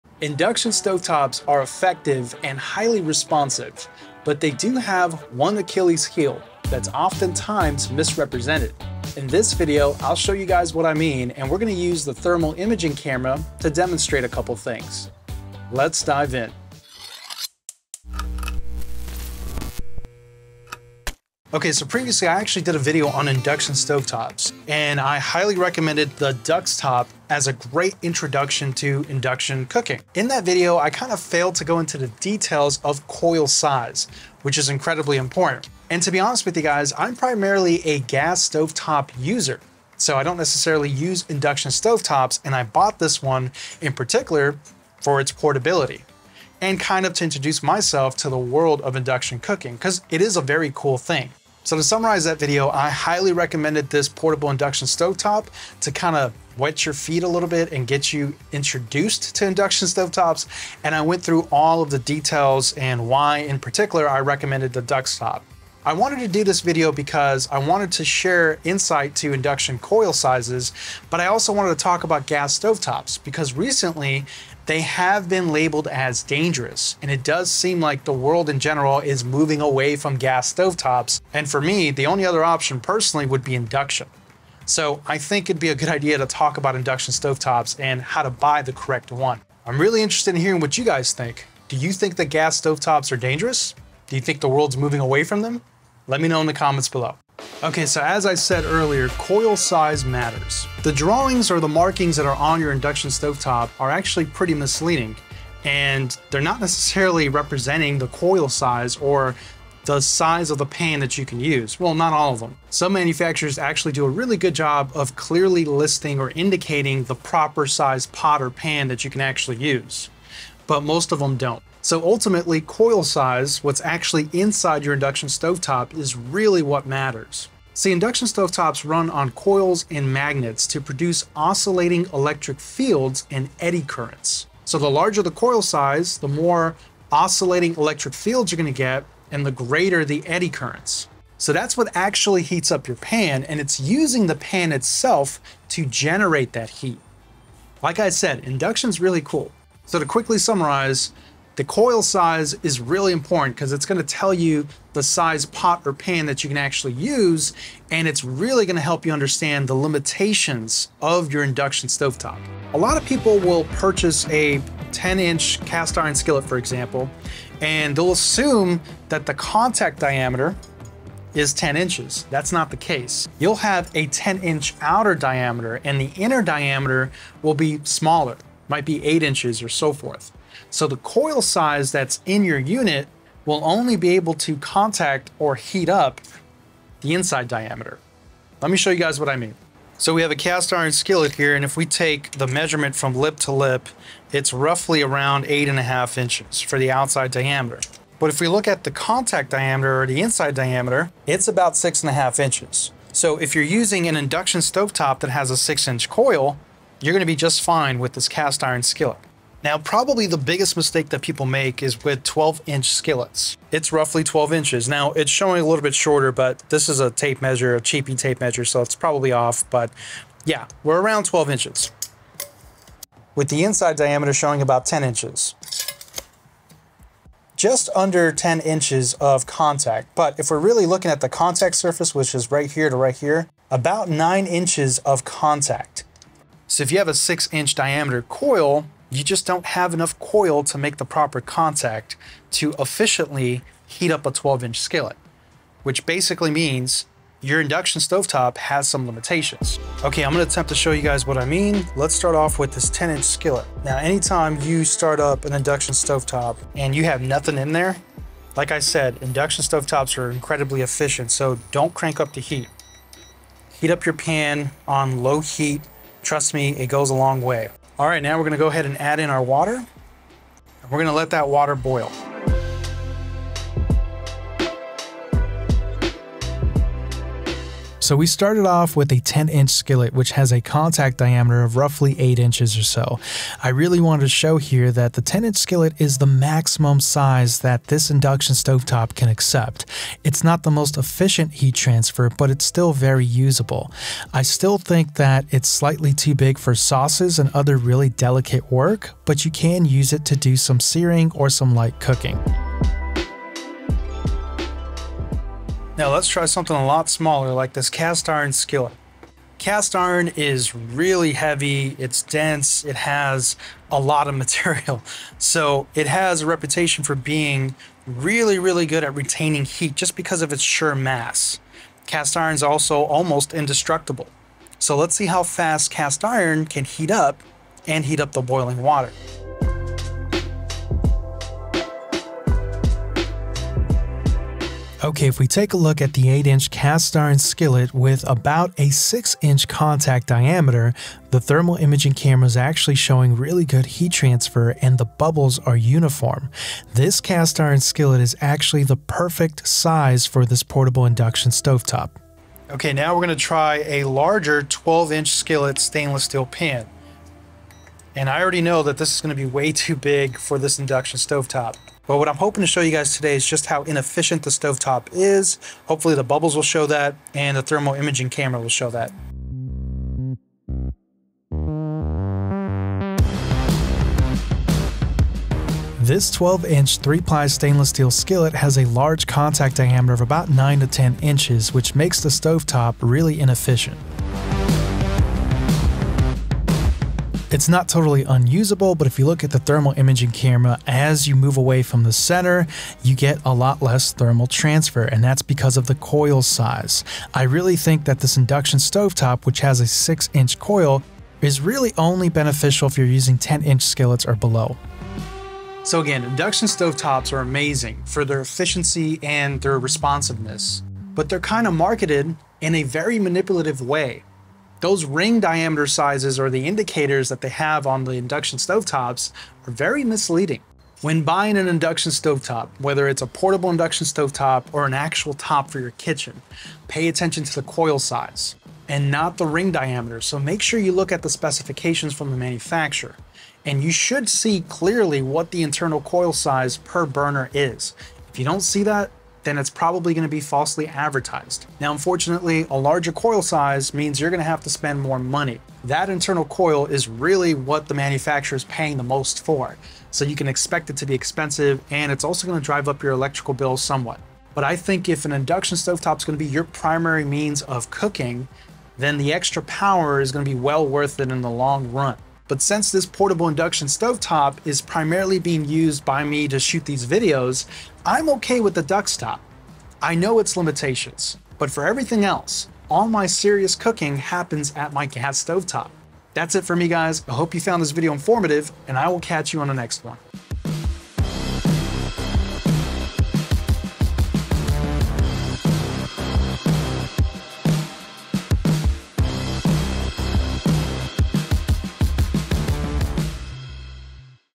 Induction Stove Top Coil Size Matters // Thermal Imaging Shows You Why
In this video, I'll talk about how induction stove tops work and why their internal coil size is the most important thing you need to consider when buying a new stove. I'll boil some water using my Duxtop portable induction stove and show you the difference when using an 8-inch, 10-inch, and 12-inch pan. The thermal imaging camera will show you the limitation of the induction stove due to its coil size.

Chapters:
00:00 - Intro
00:21 - Background
01:05 - Are Gas Stoves Dangerous?
01:37 - Coil Size Matters
02:42 - Let’s Talk about Pots and Pan Sizes
04:09 - Boiling Water Test- Thermal Imaging Camera
06:45 - Final Thoughts- Buying an Induction Stove

//Discounts:
Try Amazon Prime for 30 days
Gift Amazon Prime

//Like my sound? Try them out with this link!